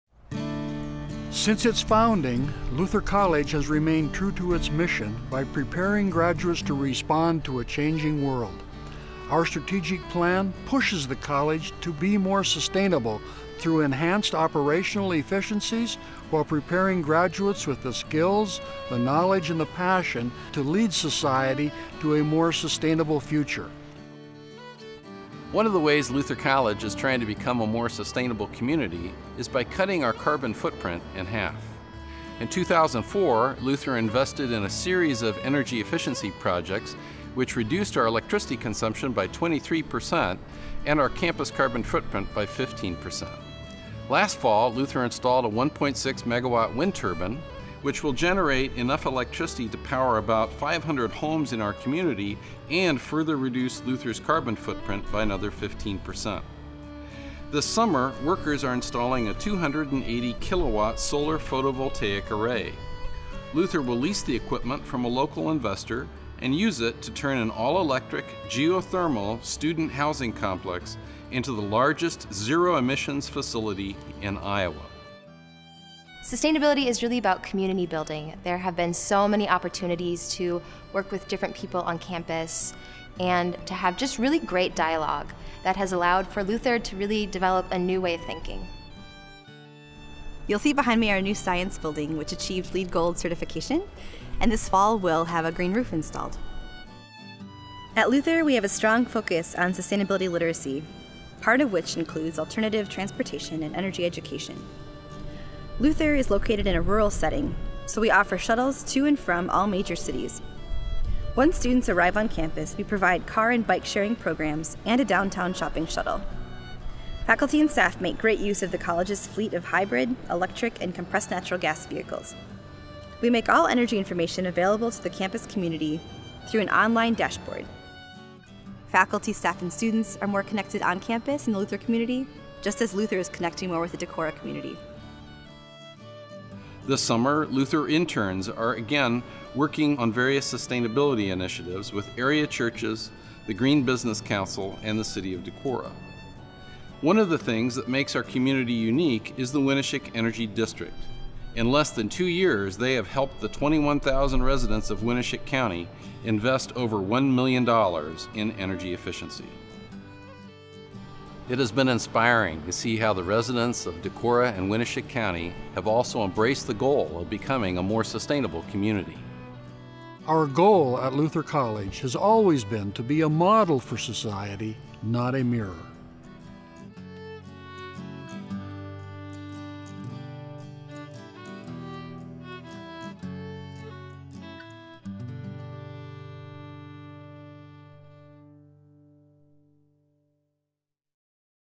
Clean Energy in Our Community: Luther College and Decorah, Iowa
The first in a new Energy.gov video series about local clean energy projects is Luther College and their partnership with the town of Decorah, Iowa.